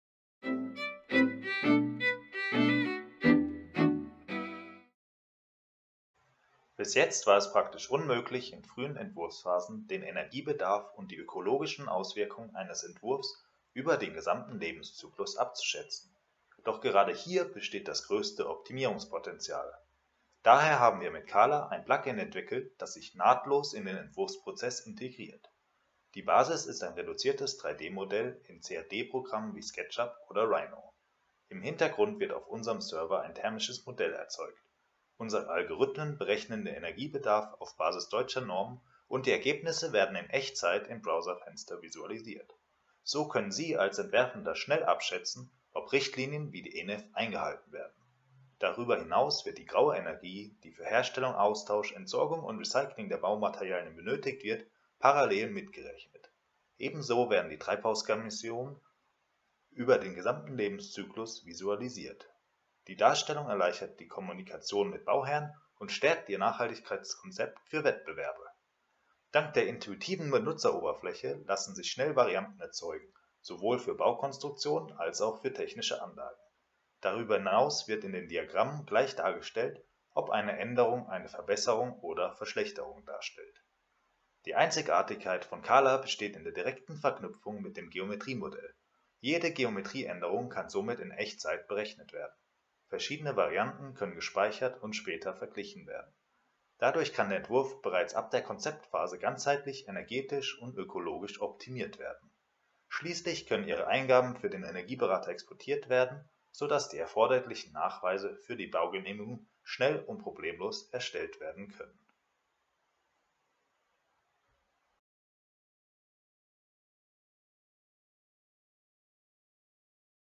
CAALA Walkthrough
Co-founder Dr. Alexander Hollberg guides you through our Sketchup and Rhino plugin CAALA In just 2 minutes, you will know how to generate live energy footprint calculations for your 3D architecture model.

Thank you for your interest in CAALA.